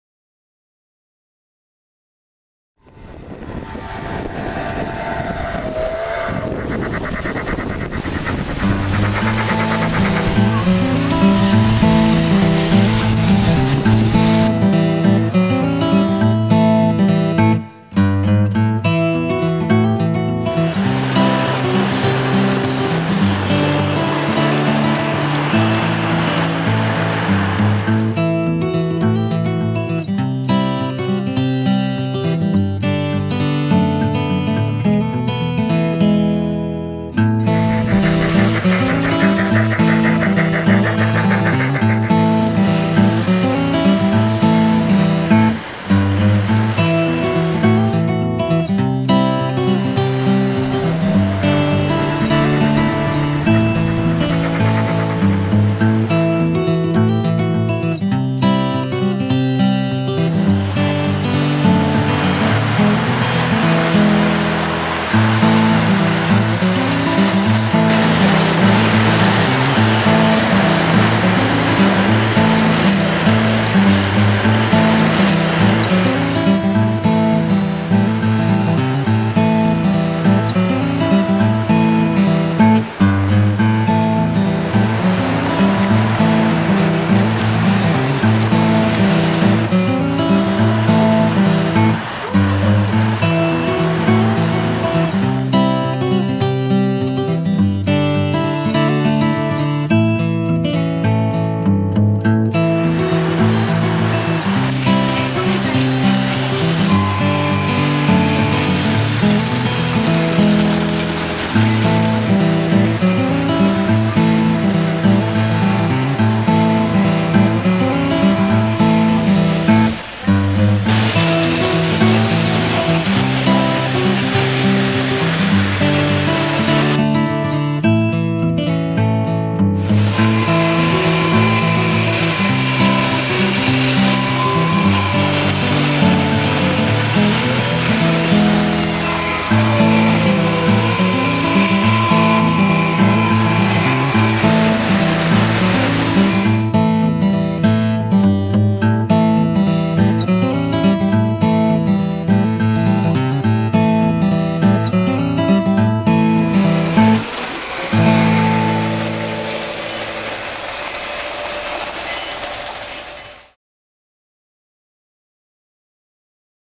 A Magical Train Day in HD
Exploring  railroad history locations and fun model train layouts near Livingston NJ